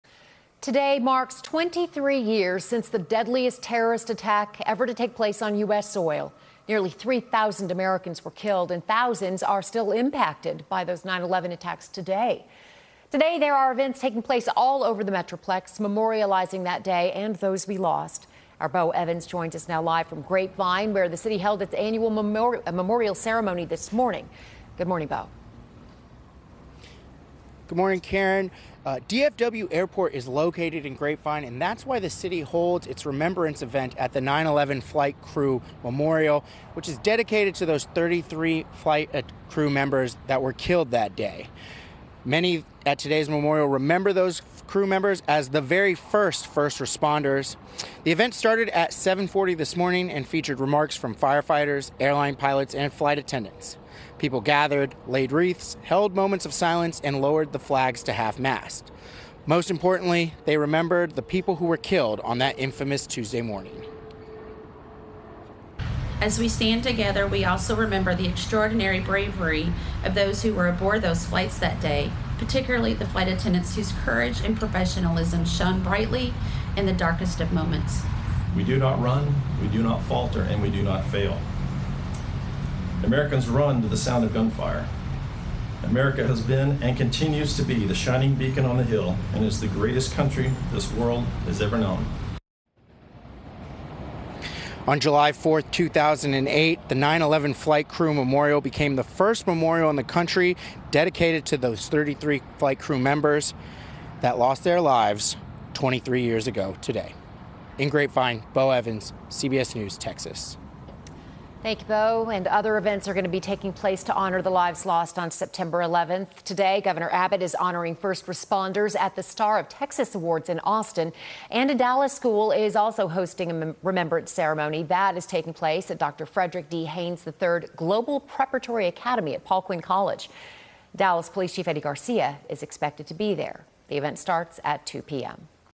9/11 commemorated at flight crew memorial in Grapevine
The 9/11 Flight Crew Memorial north of DFW International Airport in Grapevine is dedicated to the 33 airline pilots and flight attendants who were killed in the September 11 terror attacks. Bo Evans reports.